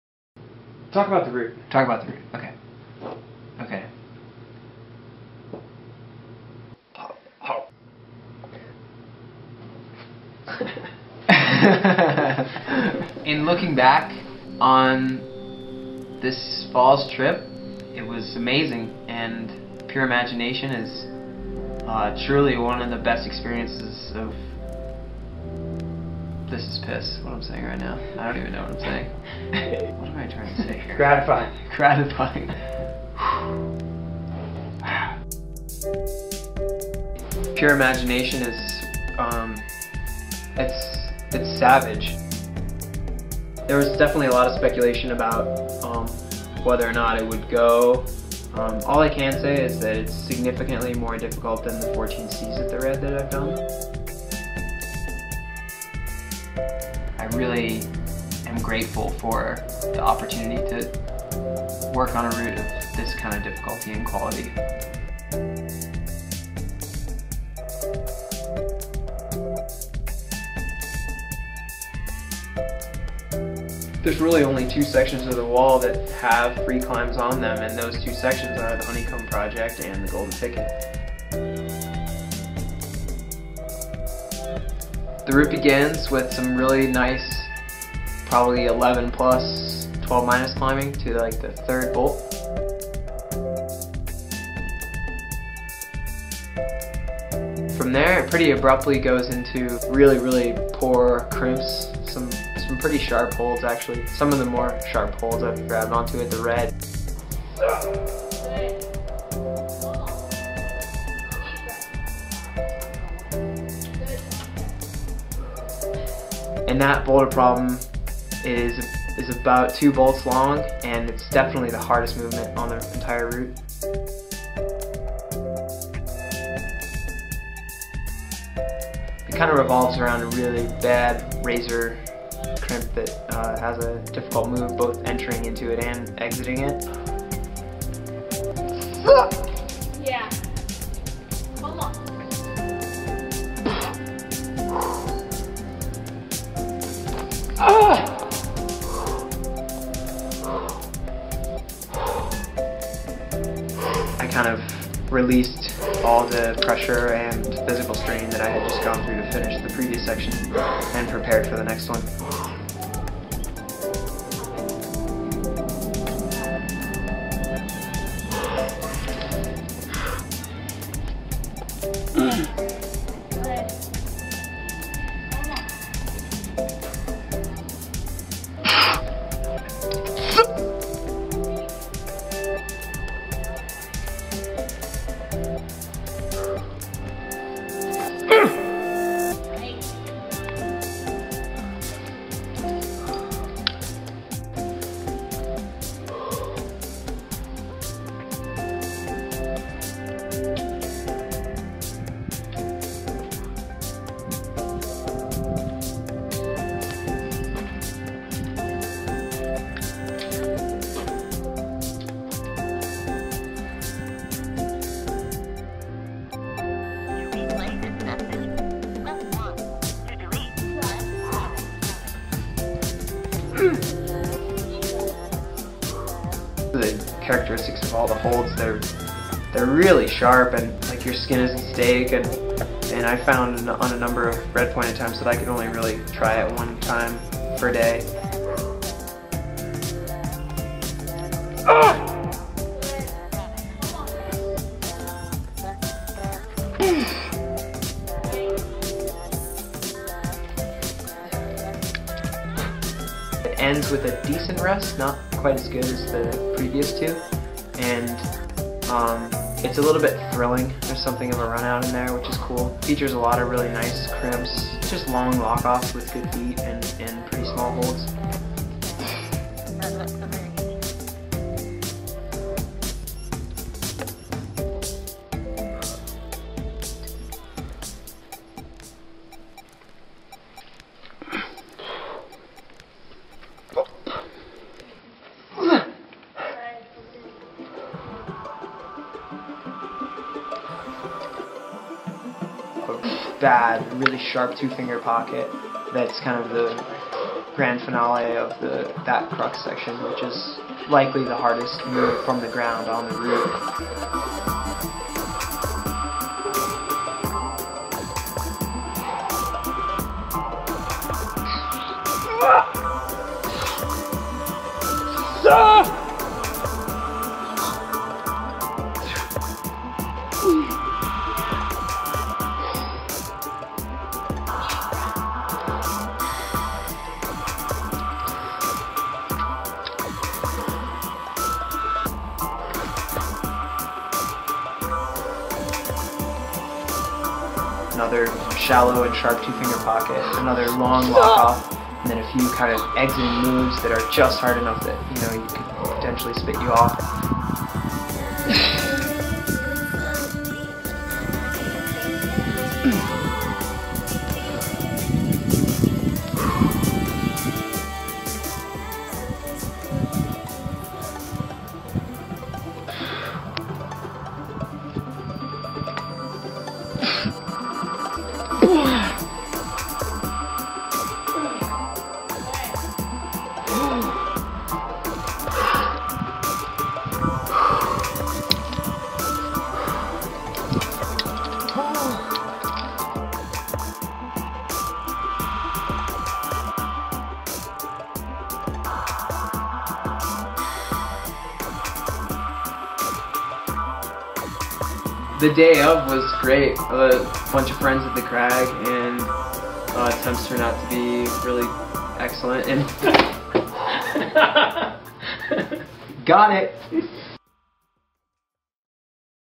ARC'TERYX Jonathan Siegrist - Pure Imagination
ARC'TERYX Athlete Jonathan Siegrist discusses his latest route "Pure Imagination" at the Red River Gorge, Kentucky. Filmed by Andy Mann.